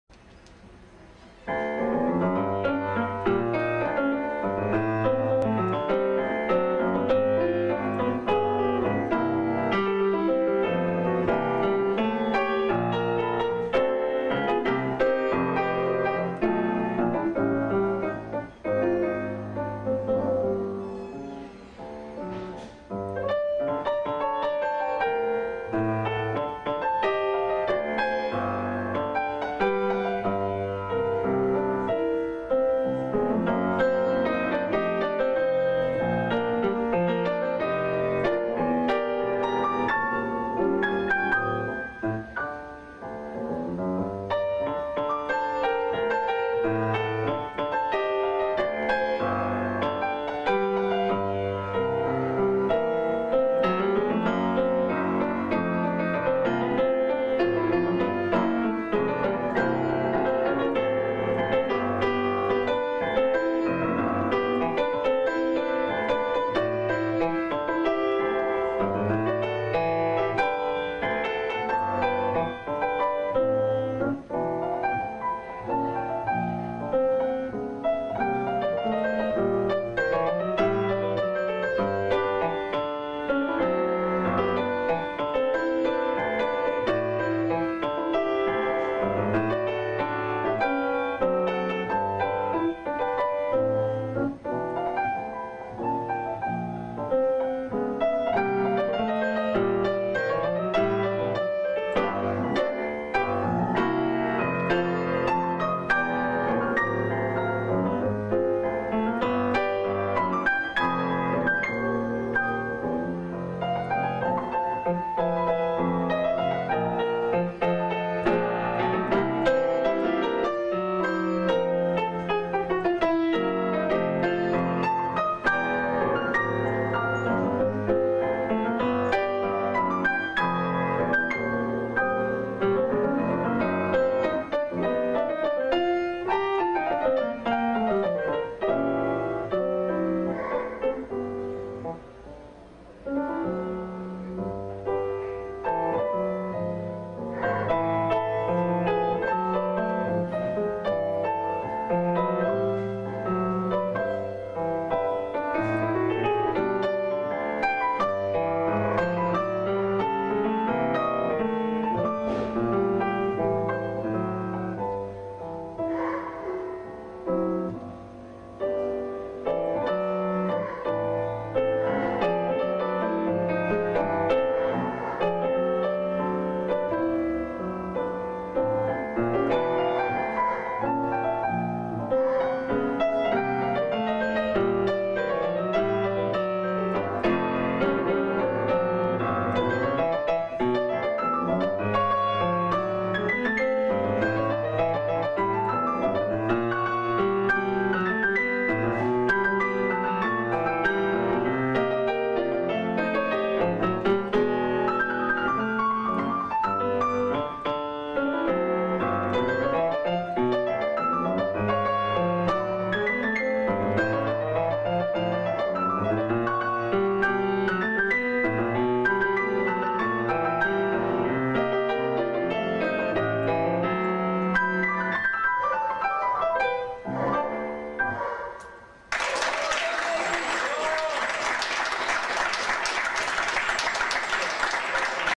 EMBA - Final de Piano 2010 - Rodrigo Caro Langwagen
Final de Piano 6 por Rodrigo Caro Langwagen en la Sede de Músicos La Pampa 2124.

Ciudad de la Paz 1831, Buenos Aires, Argentina.